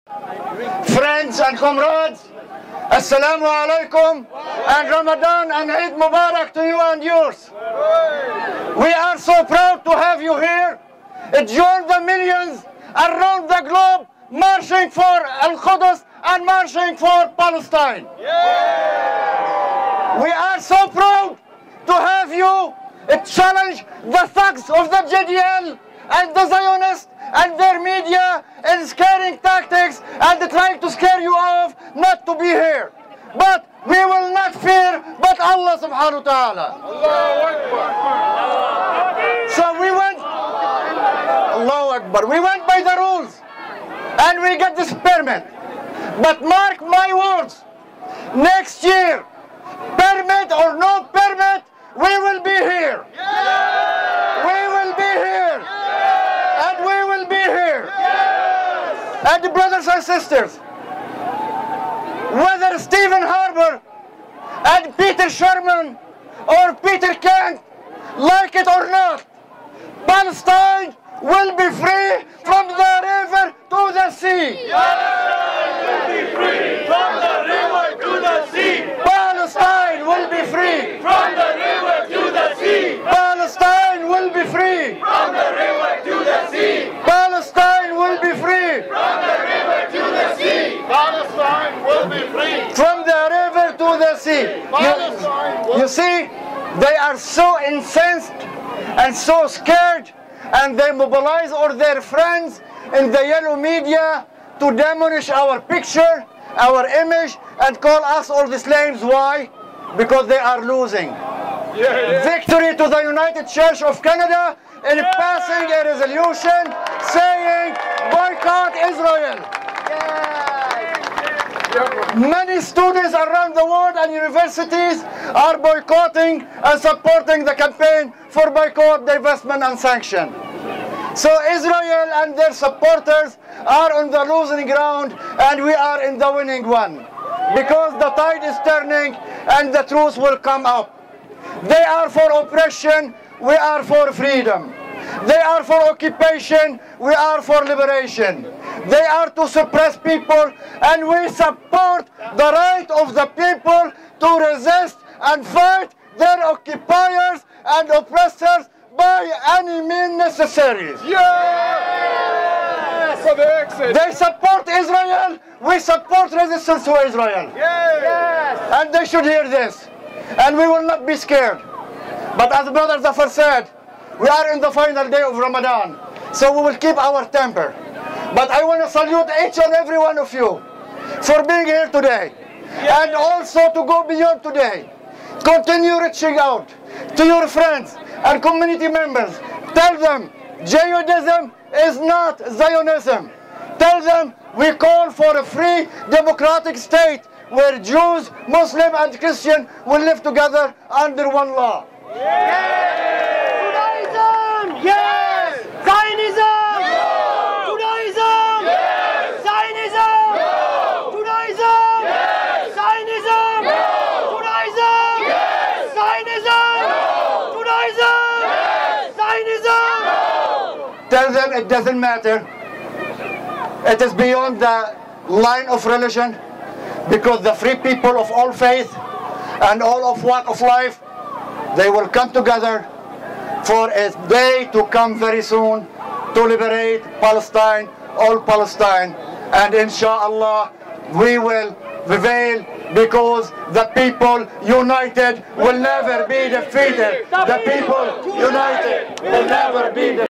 2012 Al-Quds Rally Toronto -Speech by Br. Ali Mallah - English
2012 Al-Quds Rally Toronto -Speech by Br. Ali Mallah - English